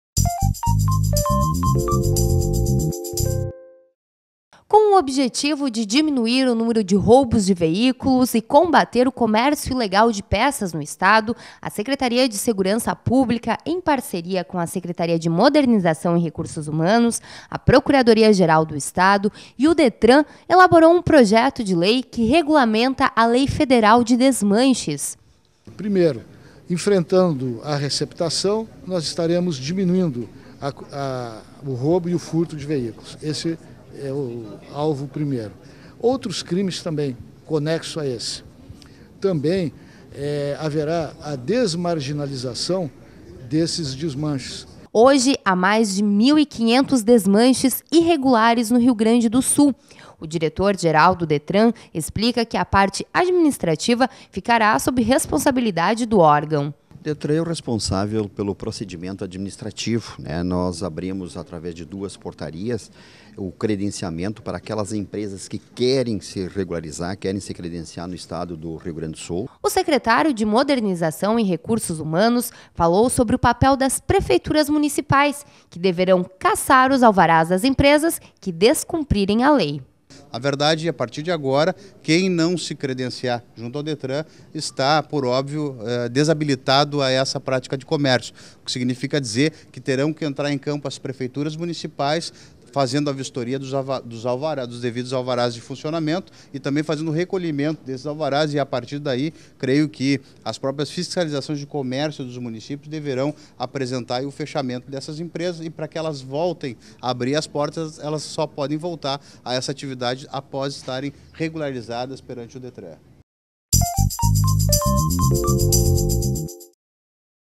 Projeto de Lei Regulamenta Fiscalização de desmanches e comércio de peças no Estado
Com o objetivo de diminuir o número de roubos de veículos e combater o comércio ilegal de peças no estado, a Secretaria de Segurança Pública, em parceria com a Secretaria de Modernização e Recursos Humanos, a Procuradoria-geral do Estado e o Detran, elaborou um projeto de lei que regulamenta a lei federal de desmanches.